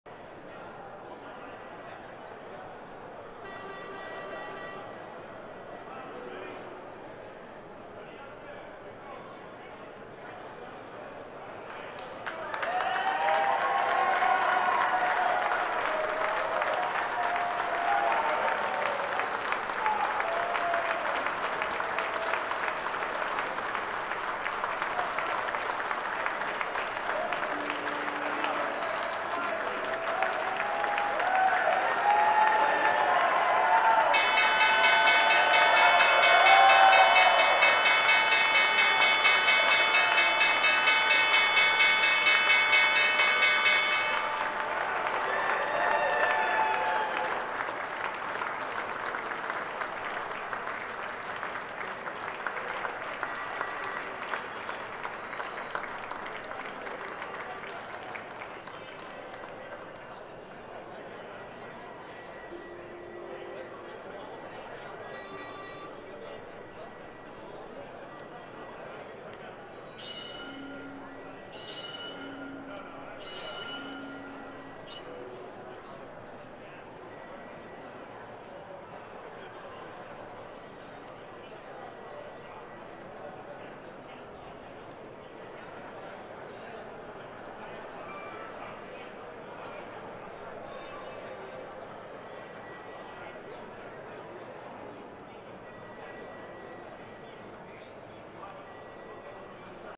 MetLife celebrates 150 years since their founding by ringing the NYSE Opening Bell
The New York Stock Exchange welcomes MetLife, Inc. Celebrating the company’s 150th Anniversary of Founding Steven Kandarian of MetLife rings the NYSE Opening Bell.